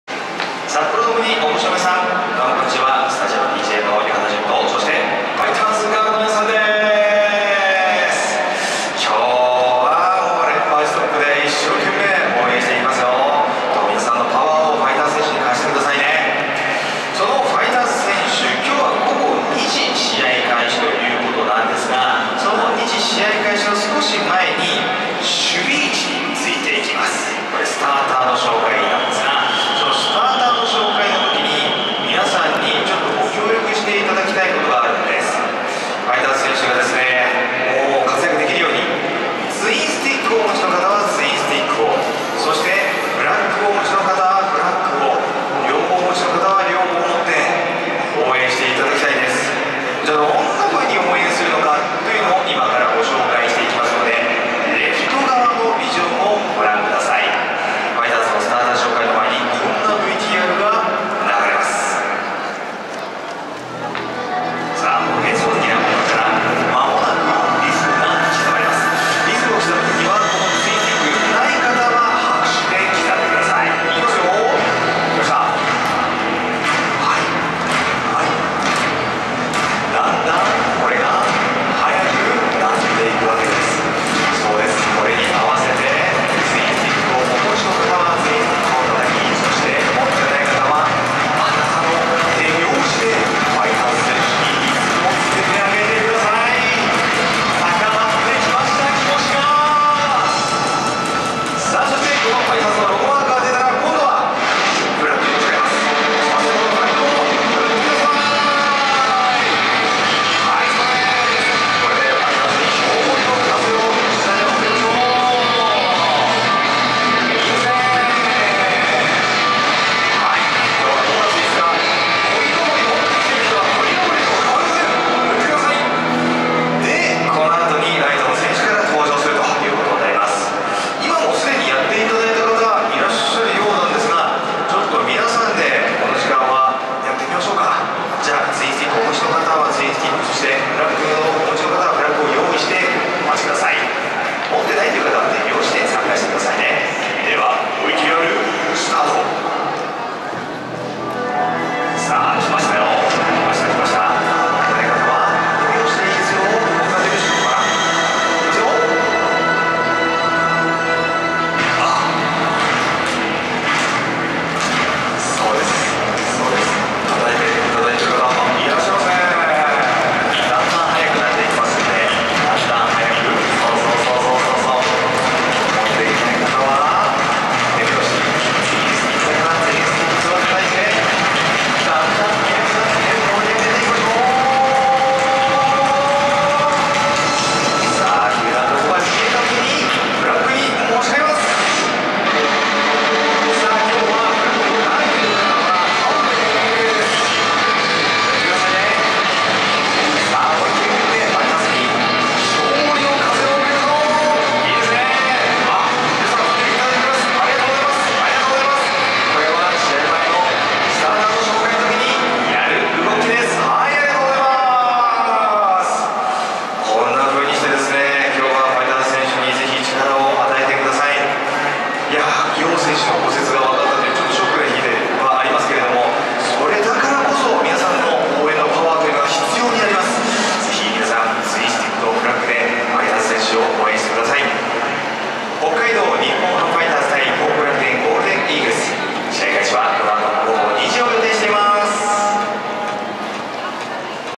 150505 ファイターズ スタメン登場時の応援レクチャー(八幡 菊池 菅又 浜野篇)
2015年4月29日、札幌ドームでの北海道日本ハムファイターズ公式戦の試合前に行われた、スタメン登場時の応援レクチャーです。

スタジアムDJ 八幡淳さん
ファイターズガール 菊池綾さん、菅又千夏さん、浜野佑香さん